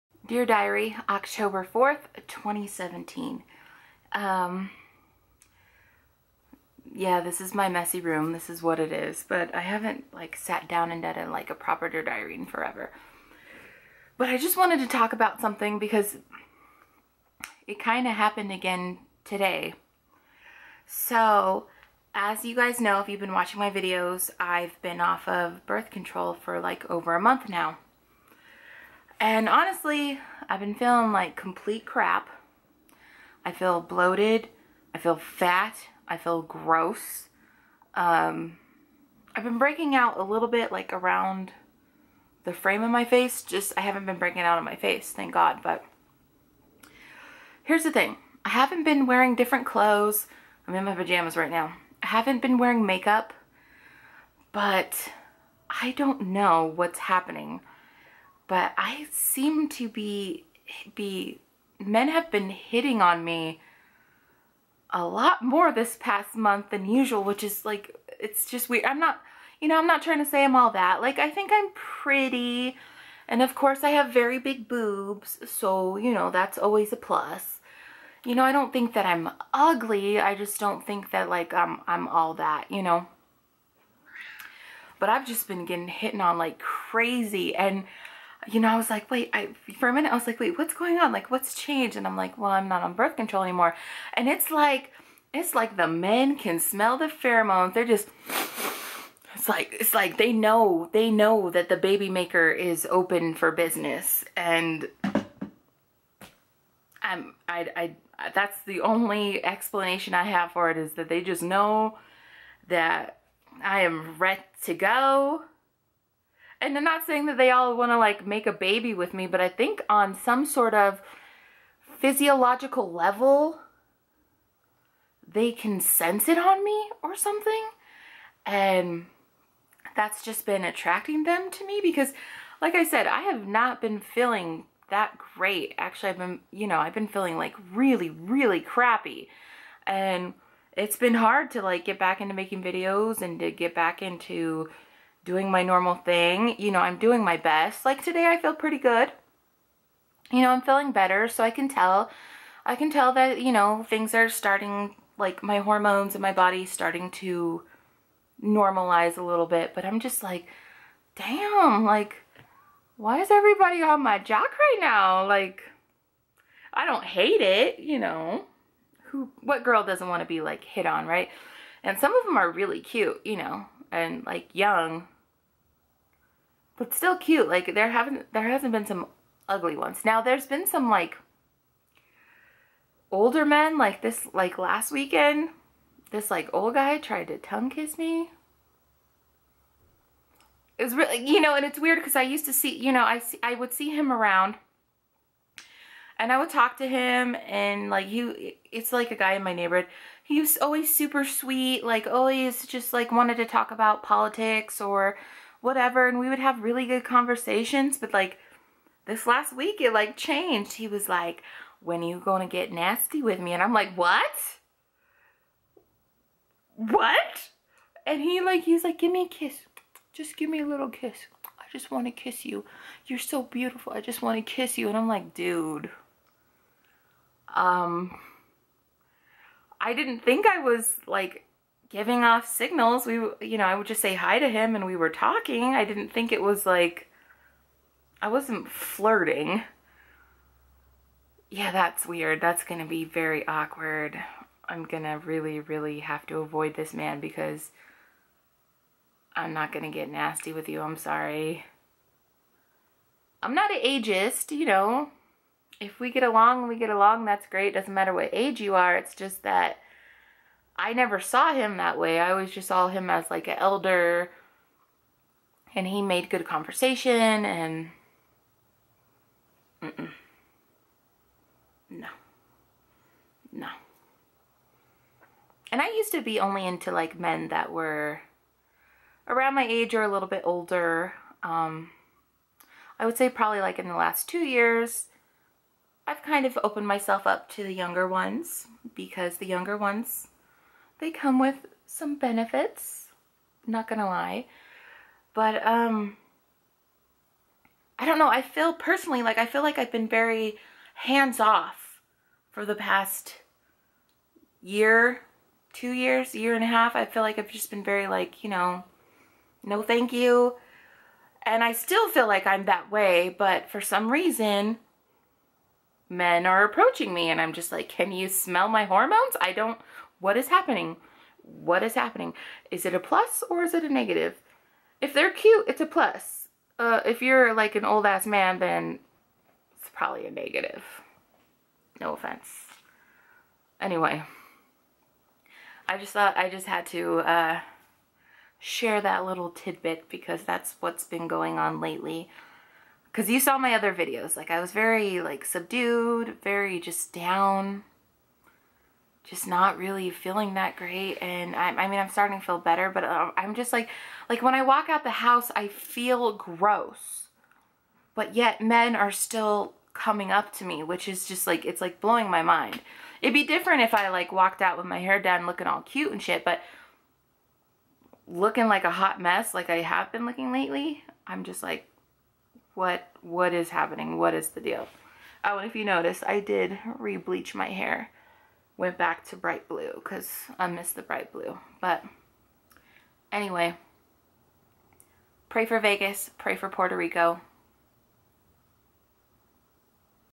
DEAR DIARY | THE MEN CAN SMELL MY PHEROMONES
Just another chat/story time of what ive been experiencing lately.

CAMERAS USED:
   Vlog Cameras:  Sony HandyCam PJ275 (60fps)
                               Canon G9X (60fps)
   Sit Down Videos: Gopro Hero4 silver (60fps)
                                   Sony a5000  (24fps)